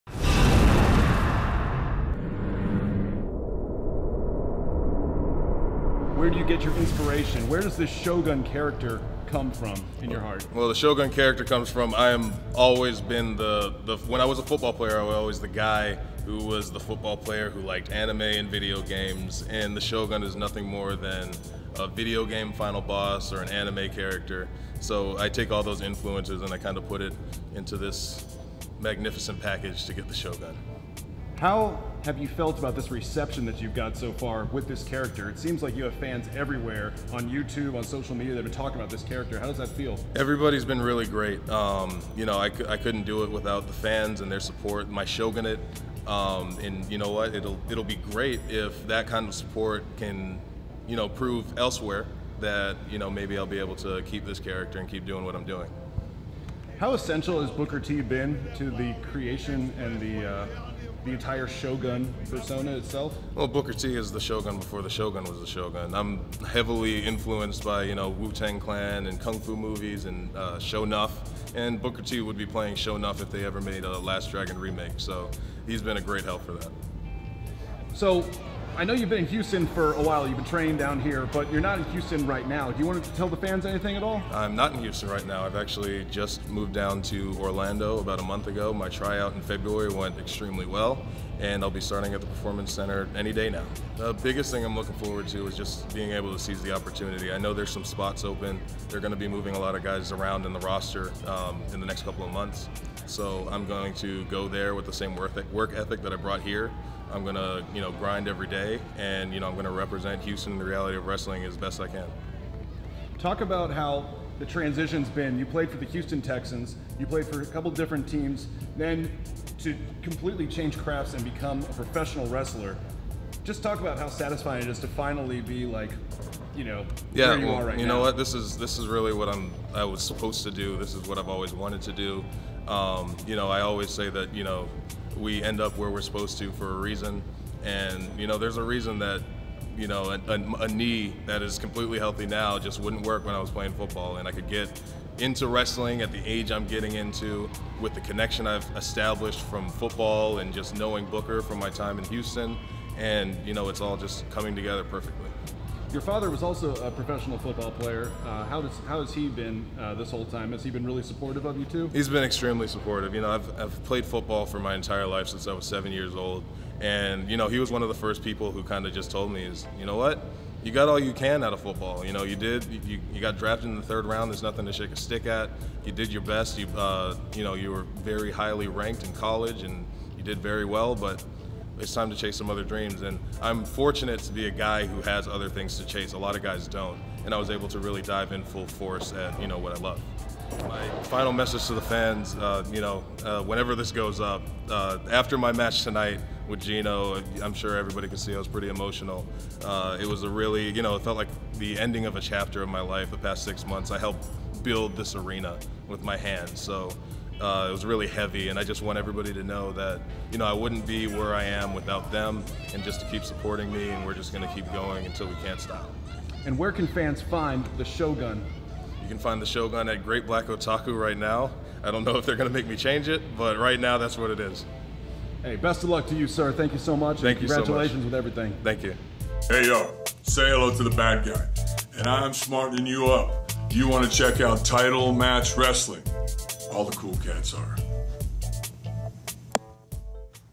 Brennan Williams (Marcellus Black) Signs with NXT
Exclusive: WWE just signed former Houston Texans player Brennan Williams aka Marcellus Black! The 6'7 286lbs Shogun has been training with Booker T at Reality of Wrestling for the greater part of 2016. Title Match Wrestling caught up with Marcellus Black (real name Brennan Williams) just moments after his final battle in the ROW.

WWE just wait because the Shogun on the ROW will soon be the Shogun of NXT! Follow

We at Title Match Wrestling would like to send a shout out & big thank you to Booker T, Sharmell and the entire ROW crew for working so hard on making Reality of Wrestling the flagship of Texas wrestling!

Come see the ROW Live!